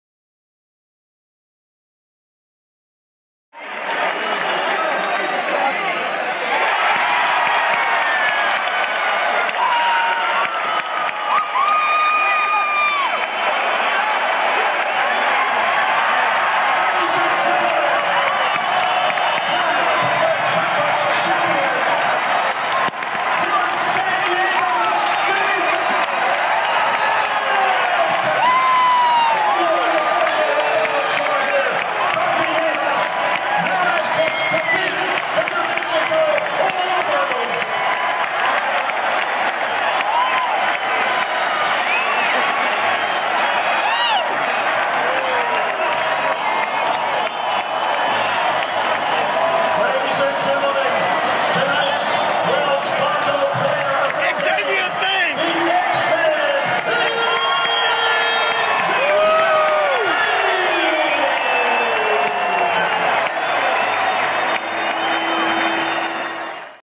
San Diego Aztecs Win the Mountain West Championship
Aztecs Win the Mountain West Championship
Coming back from 25-41 midway through the second half to Win 51-48
The Student Body AKA "THE SHOW" Rushes the court to celebrate.